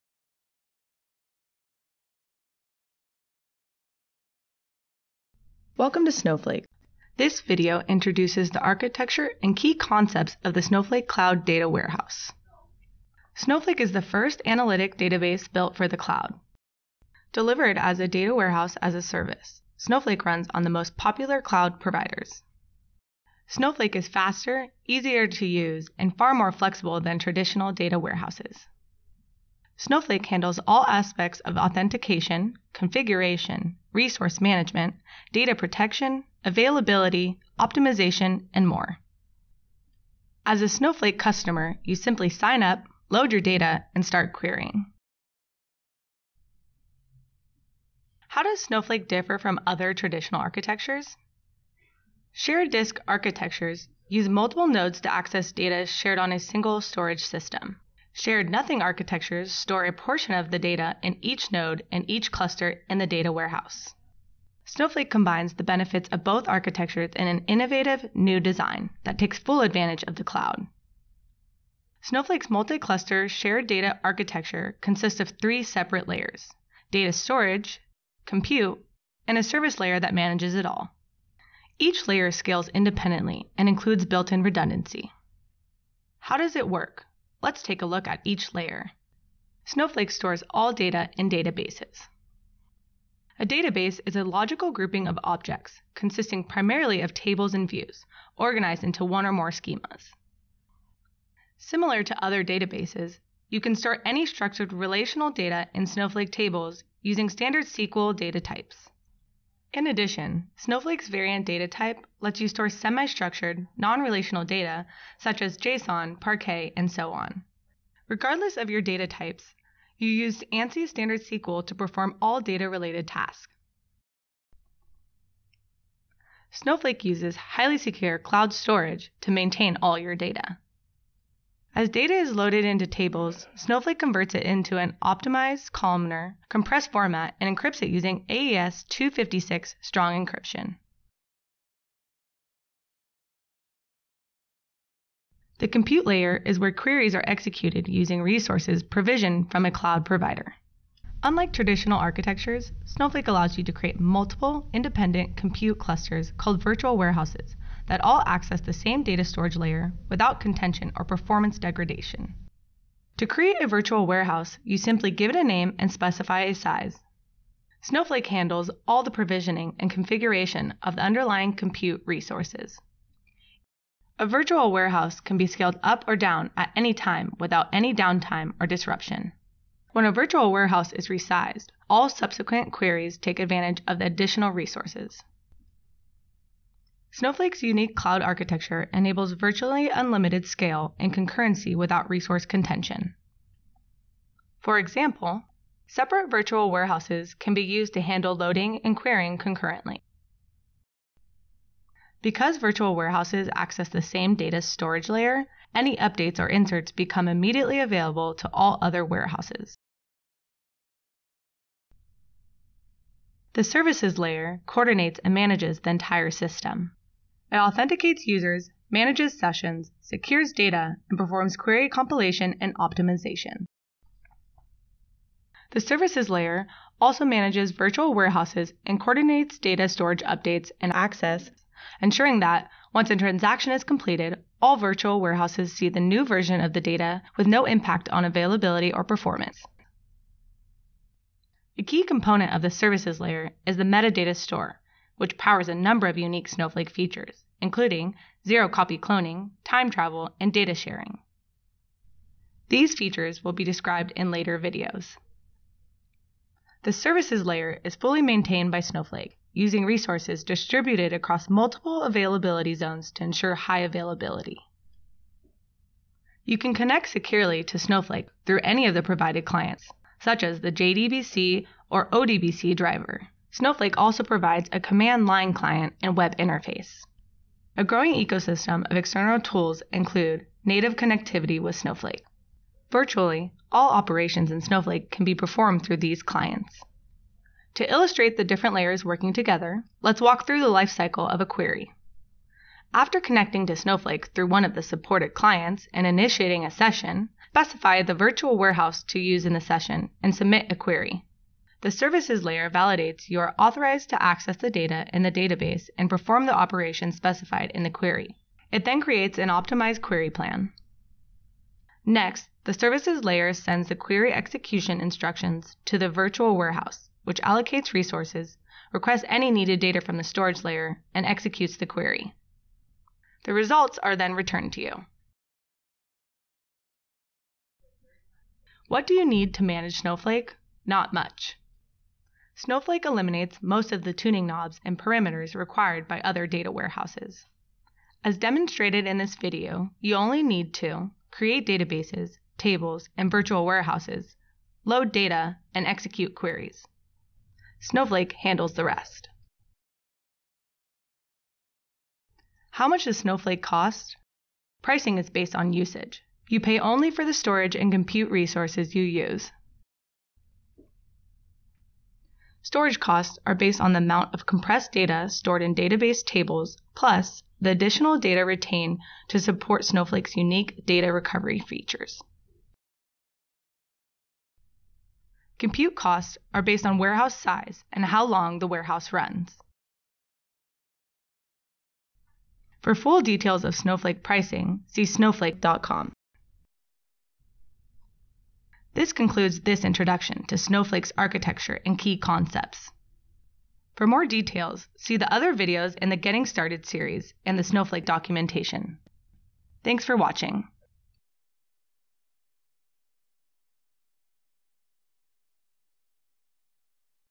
Getting Started - Architecture & Key Concepts
This video provides a high-level overview of the key concepts used in Snowflake and the major components of Snowflake's multi-cluster, shared-data architecture.

Be sure to visit the Snowflake Developers YouTube Channel for more technical videos: